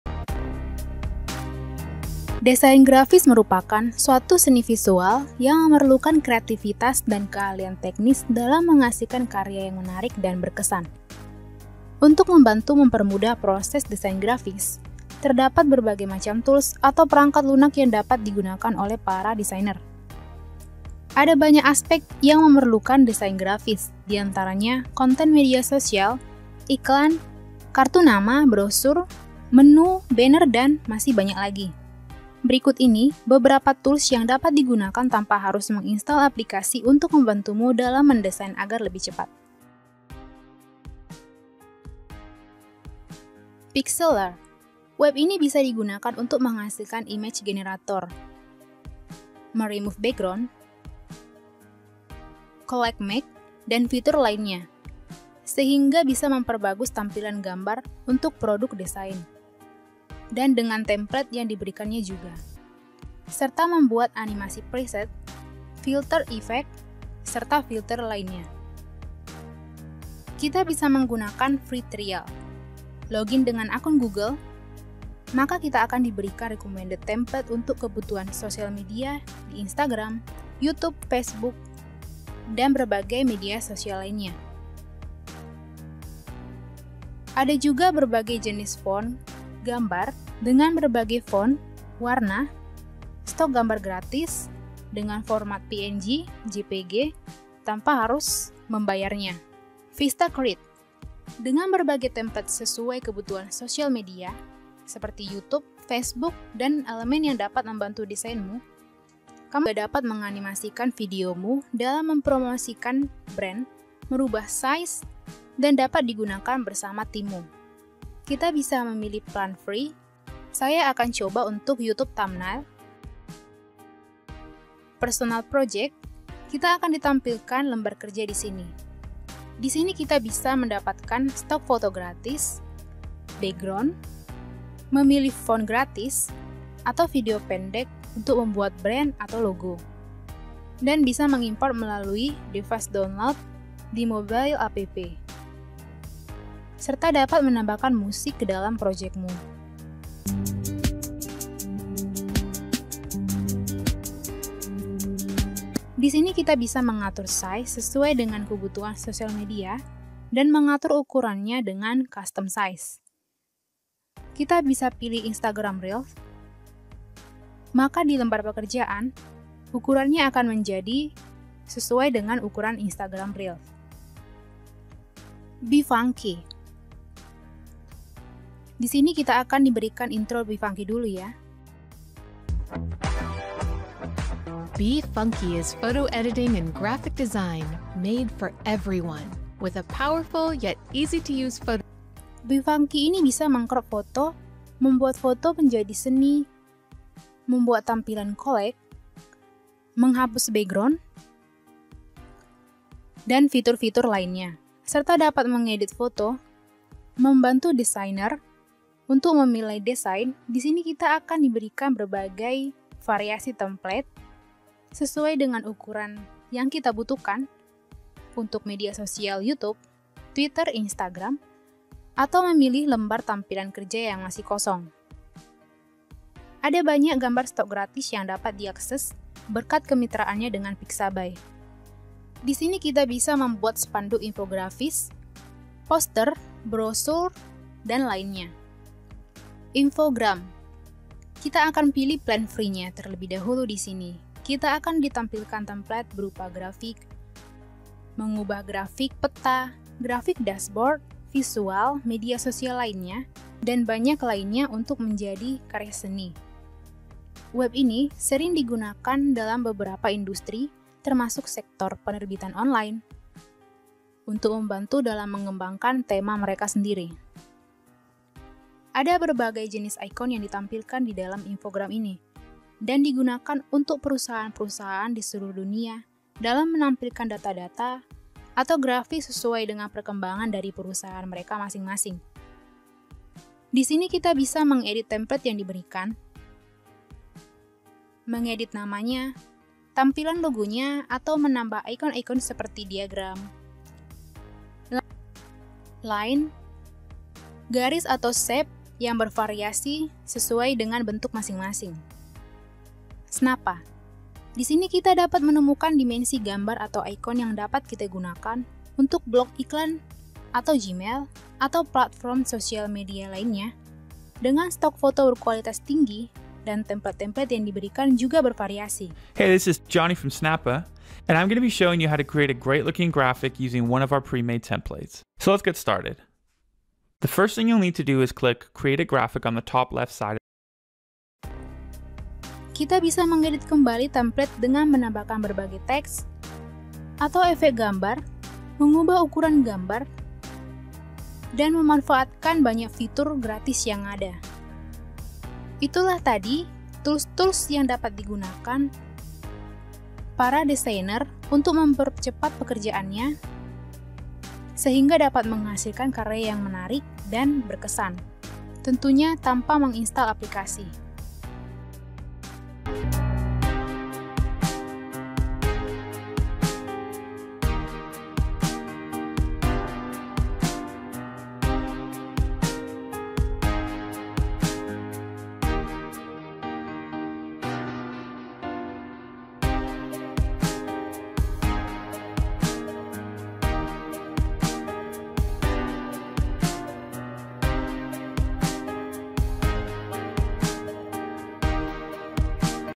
5 Tools Desain Grafis free dan berbayar untuk design lebih keren
Desain Grafis Merupakan Suatu Seni Visual Yang Memerlukan Kreativitas Dan Keahlian Teknis Dalam Menghasilkan Karya Yang Menarik Dan Berkesan. Untuk Membantu Mempermudah Proses Desain Grafis, Terdapat Berbagai Macam Tools Atau Perangkat Lunak Yang Dapat Digunakan Oleh Para Desainer.

Berikut ini beberapa tools yang dapat digunakan tanpa harus  menginstall aplikasi  untuk membantu mu dalam  mendesign  agar lebih cepat